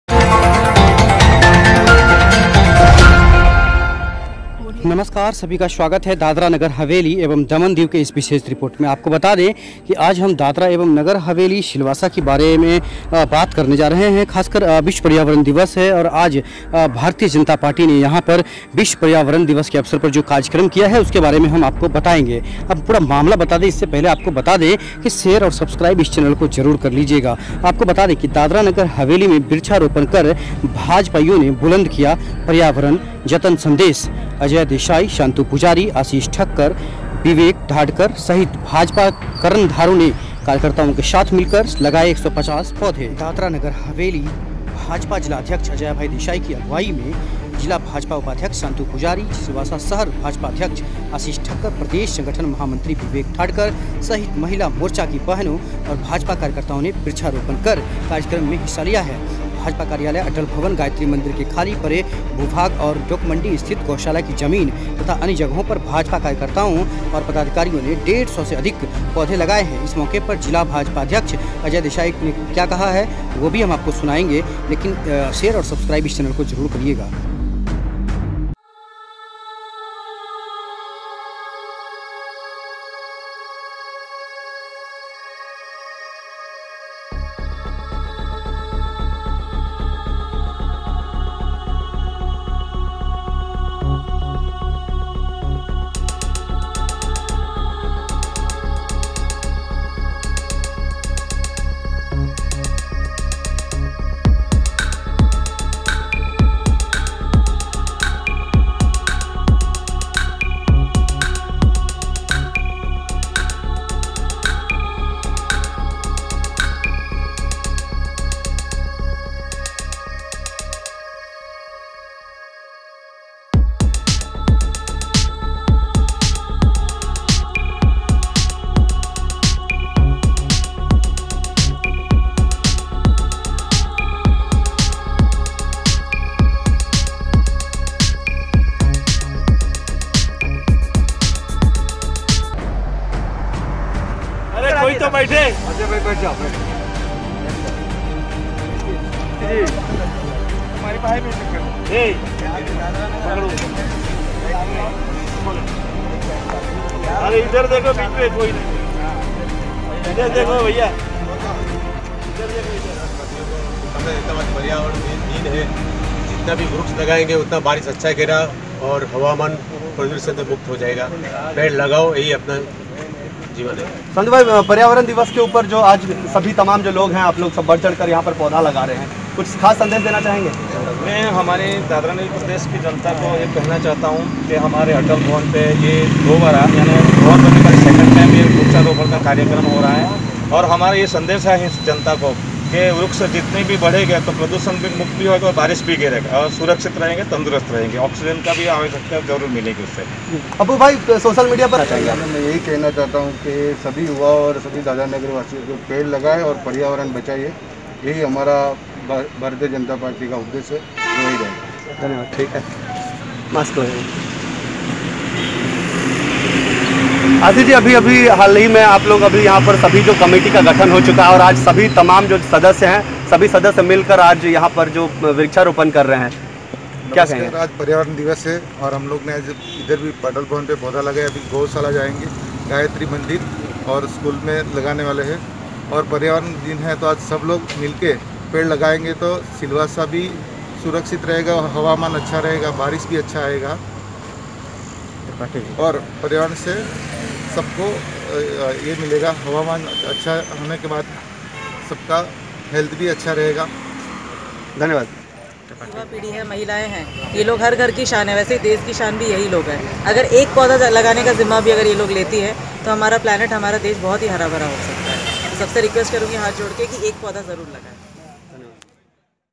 दादरा एवं नगर हवेली भाजपा के जिला अध्यक्ष अजय भाई देसाई के नेतृत्व में वृक्षारोपण किया गया
*दानह में वृक्षारोपण कर भाजपाईयों ने बुलंद किया पर्यावरण जतन संदेश

■ *अजय देसाई,शांतु पुजारी,आशीष ठक्कर,विवेक दाढ़कर सहित भाजपा कर्णधारों ने कार्यकर्ताओं के साथ मिलकर लगाये 150 पौधे

 पर्यावरण दिवस पर दादरा नगर हवेली में भारतीय जनता पार्टी ने वृक्षारोपण कर पर्यावरण का जतन करने का संदेश दिया। दानह भाजपा जिलाध्यक्ष अजय देसाई की अगुवाई में जिला भाजपा* *उपाध्यक्ष शांतु पुजारी, सिलवासा शहर भाजपाध्यक्ष आशीष ठक्कर,प्रदेश संगठन महामंत्री विवेक दाढ़कर सहित महिला मोर्चा की बहनों और भाजपा कार्यकर्ताओं ने वृक्षारोपण कार्यक्रम में हिस्सा लिया।* *भाजपा कार्यालय अटल भवन,गायत्री मंदिर के खाली पड़े भू-भाग और डोकमरडी स्थित गौशाला की जमीन तथा अन्य जगहों पर भाजपा कार्यकर्ताओं और पदाधिकारियों ने डेढ़ सौ पौधे रोपे। इस मौके पर जिला भाजपाध्यक्ष अजय देसाई का कहना था कि एक वृक्ष को दस पुत्रों के समान कहा गया है। सभी लोगों को वृक्षारोपण करना चाहिए। जितने अधिक वृक्ष होंगे उतना ही उत्तम पर्यावरण होगा। जिला उपाध्यक्ष शांतु पुजारी ने कहा कि हम लोग हमेशा से पर्यावरण संरक्षण के लिए प्रयत्नशील रहे हैं। सभी को अपने आस-पास खाली जमीन पर पौधा लगाना चाहिए। दूसरों को भी ऐसा करने को प्रेरित करें। आशीष ठक्कर ने पर्यावरण दिवस पर सभी को पौधा लगाने और पर्यावरण का जतन करने की अपील की। विवेक दाढ़कर का कहना था कि वृक्ष और वन हमारे असल हितैषी हैं। हर नागरिक का कर्तव्य है कि वो अधिक से अधिक वृक्षारोपण करे। बरसात का मौसम वृक्षारोपण के लिए सबसे उप्युक्त समय होता है। हम सभी को बारिश में पौधे लगाने चाहिए* *और पौधों की यत्नपूर्वक देखभाल करनी चाहिए जब तक कि वो बढ़कर बड़े वृक्ष ना बन जायें।*